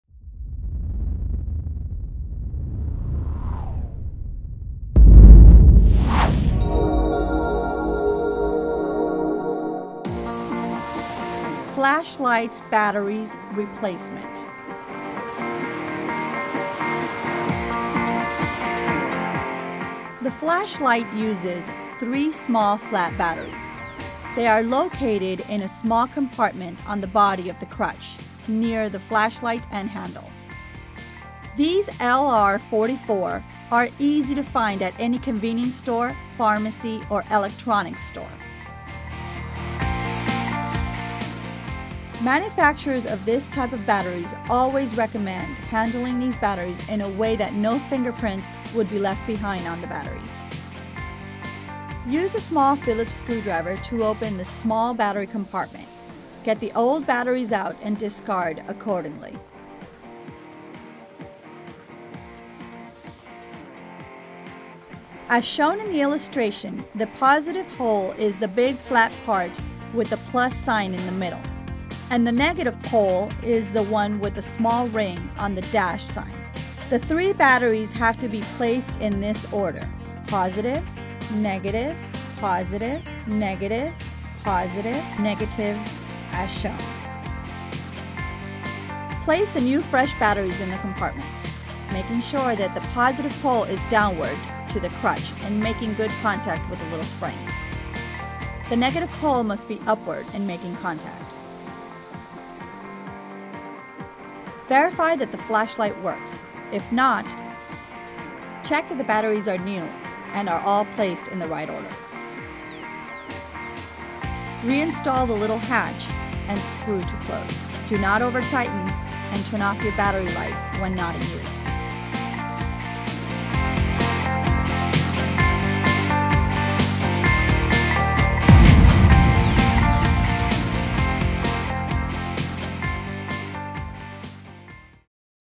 ERGOBAUM: Flashlight Batteries Replacement (Maintenance)
How to replace the flashlight batteries on your ERGOBAUM crutch.